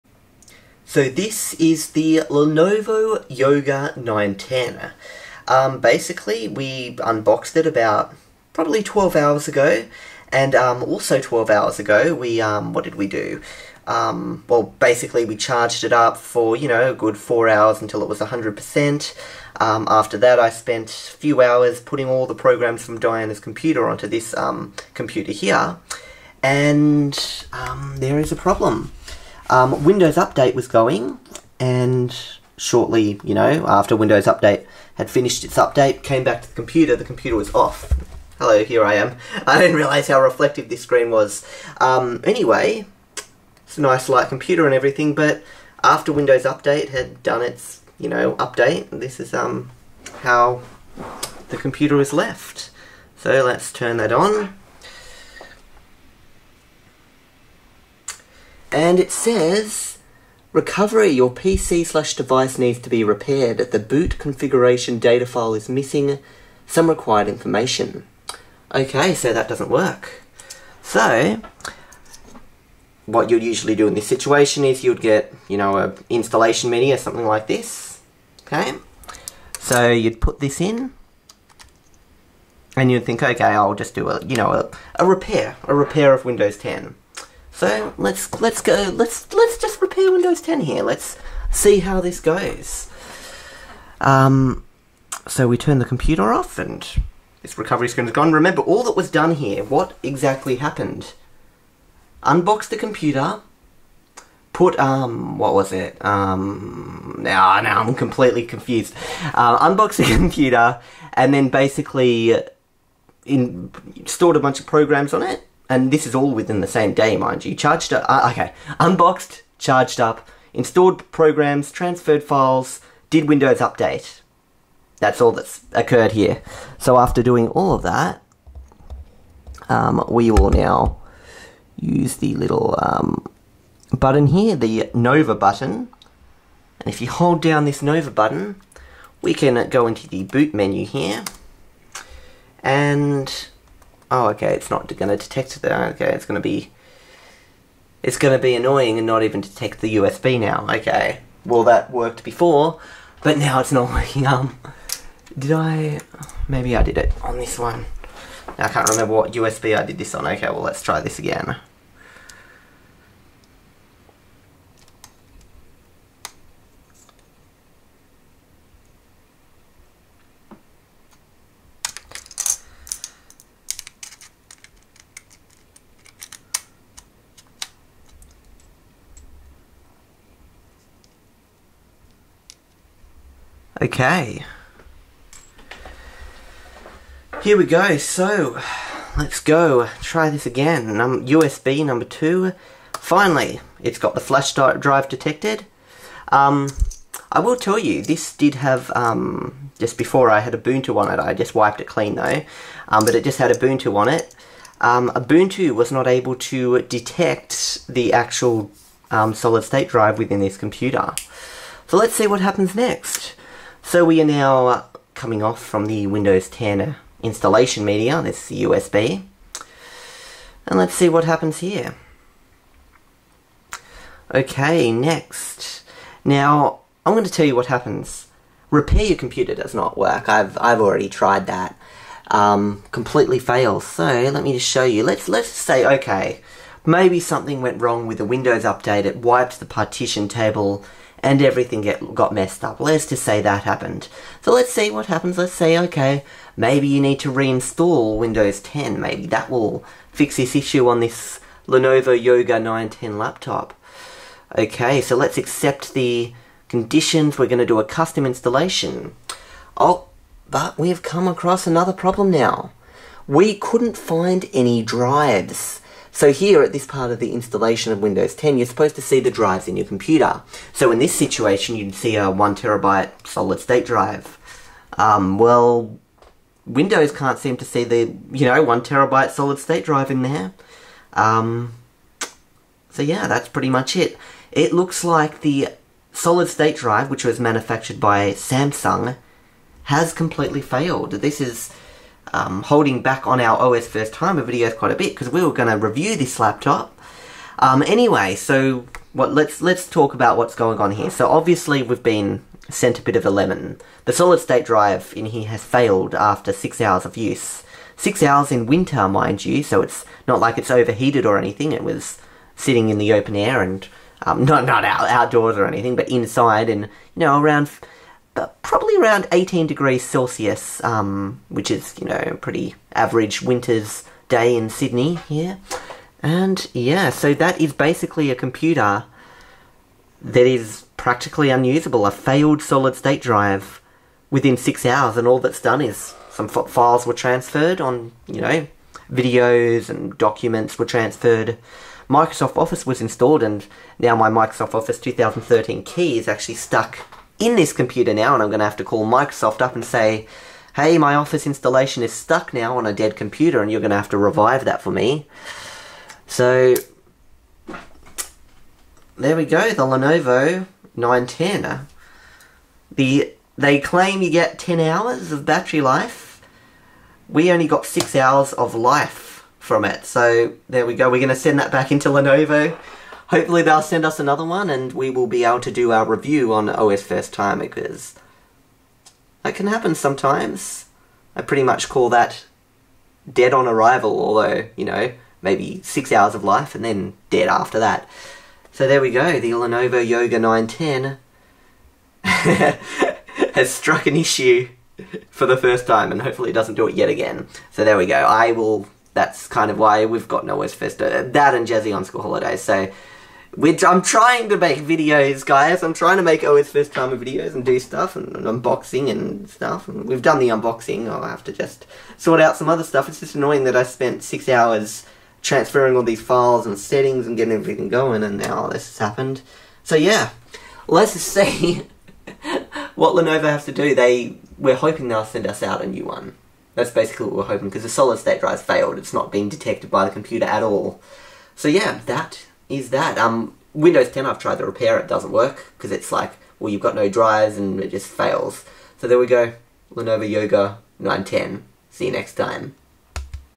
OSFT VLog | July 13th 2017 - Our Silver Lenovo Yoga 910 1TB Dies On Day 1
So today our Lenovo Yoga 910 arrived ready for us to spend a week with, to do a review on our main channel. So we unboxed the laptop, charged it up, transferred Diana's documents/videos/files, installed her software such as chrome, office etc and then let Windows update do its thing.

After all that I came back to a computer that had turned itself off. "Hmmm. maybe the update was finished" I thought. Well, I turned on the computer and I got a blue screen which you can see in the video. Looks like I'm calling up lenovo tomorrow to get a replacement. Sadly, this means our review will be delayed, possibly until next month.